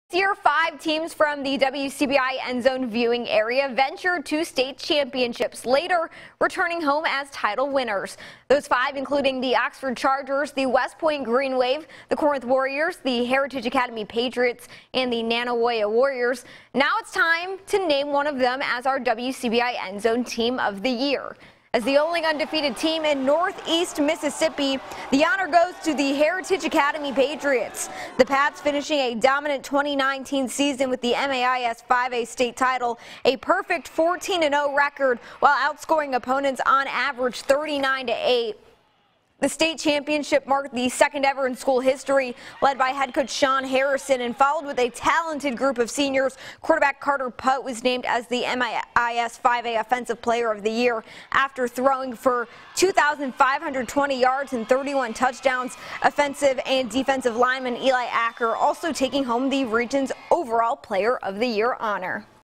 2019 EndZone Team of the Year: Heritage Academy Patriots
COLUMBUS, Miss. (WCBI) -- The only undefeated team in Northeast Mississippi takes home the honor of the WCBI EndZone Team of the Year.